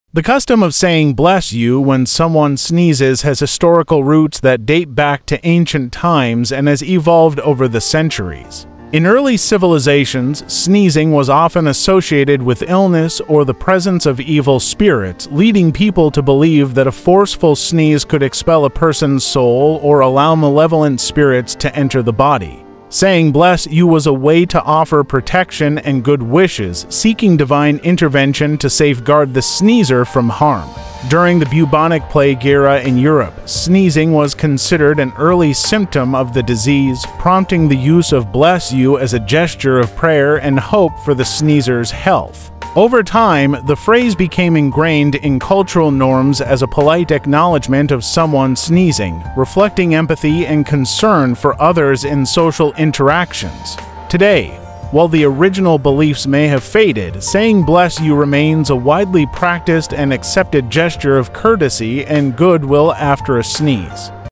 why do people say bless you after a sneeze? 🤒
Ever wonder why people say "bless you" after a sneeze? In this video, we explore the origins of this common phrase and its cultural significance. Bless you! 🤒